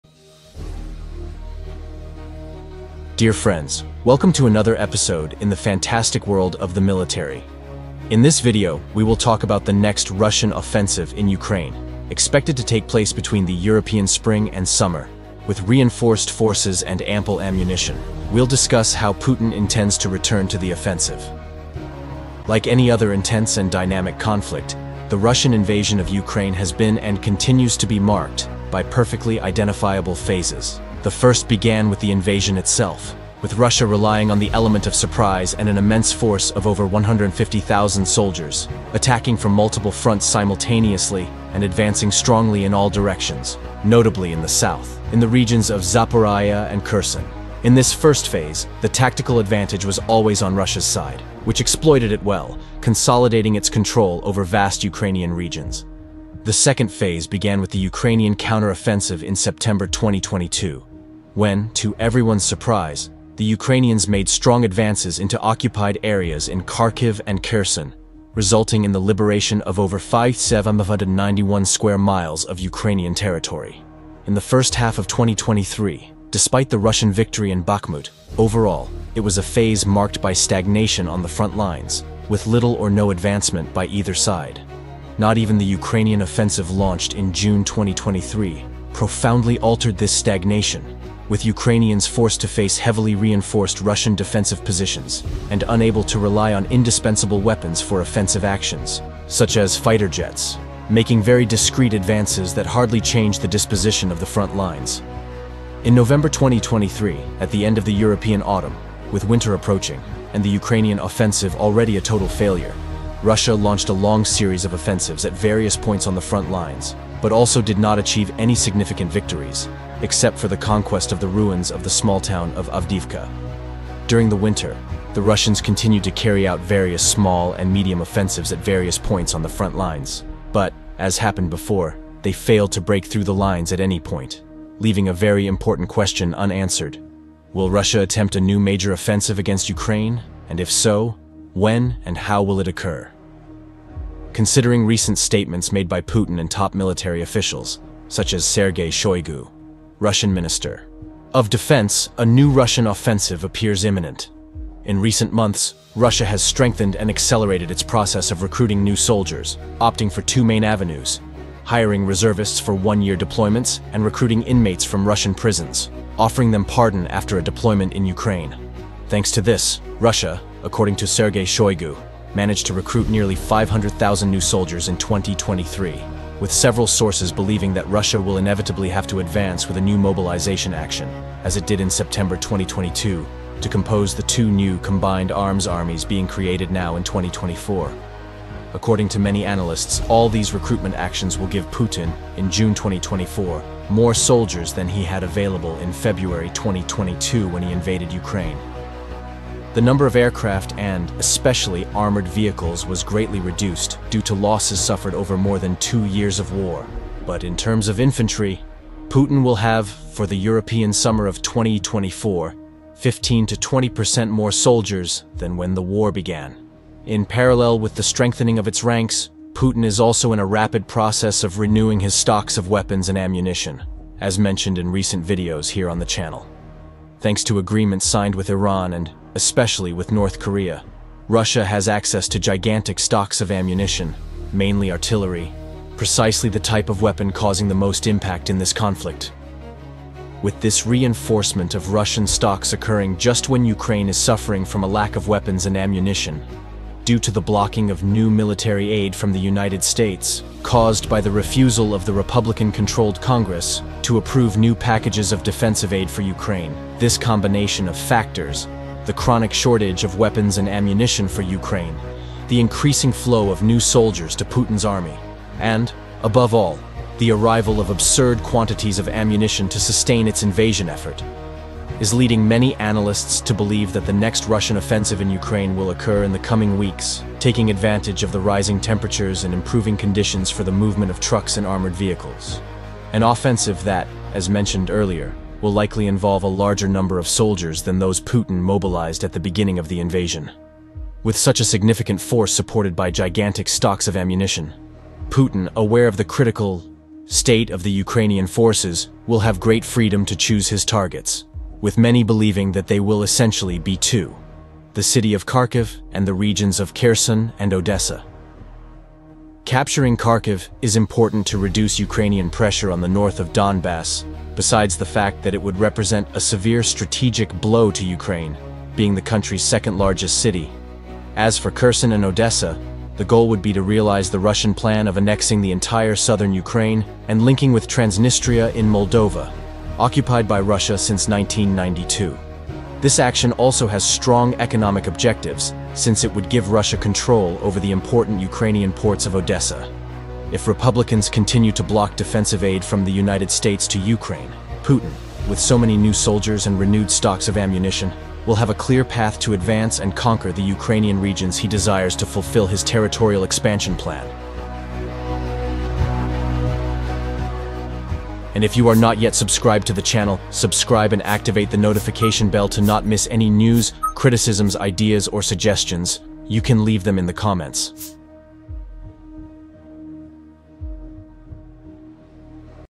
“The next Russian offensive will be devastating” What Putin’s new offensive will be like in Ukraine
Dear friends, welcome to another episode in the fantastic world of the military. In this video, we will talk about the next Russian offensive in Ukraine, expected to take place between the European spring and summer, with reinforced forces and ample ammunition. We'll discuss how Putin intends to return to the offensive.